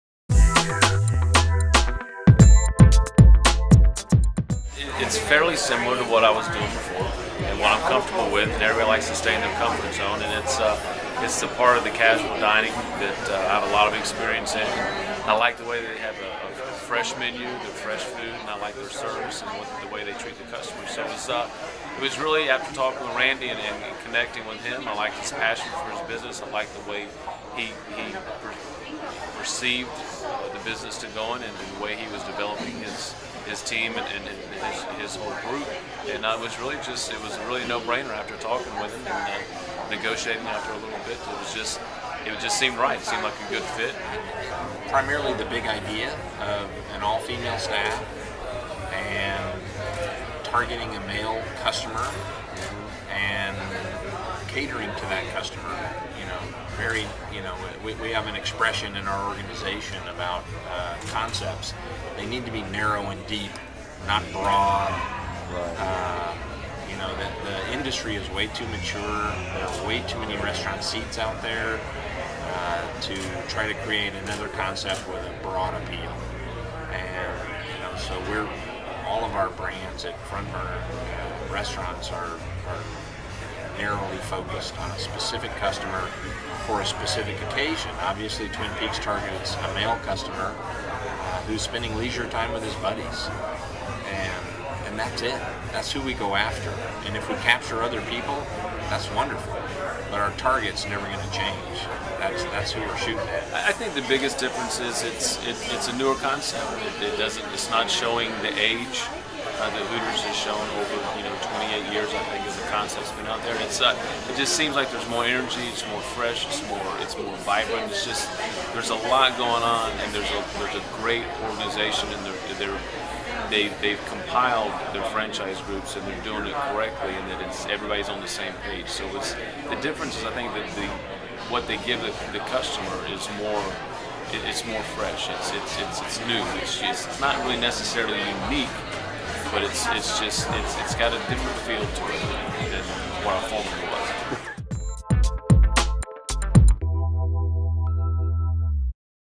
Twin Peaks taps ex-Hooters execs for franchising
Coby Brooks, former head of Hooters of America, and Randy DeWitt, co-founder of Twin Peaks, talk about their franchising deal and differences between the two "breastaurant" concepts. NRN story at: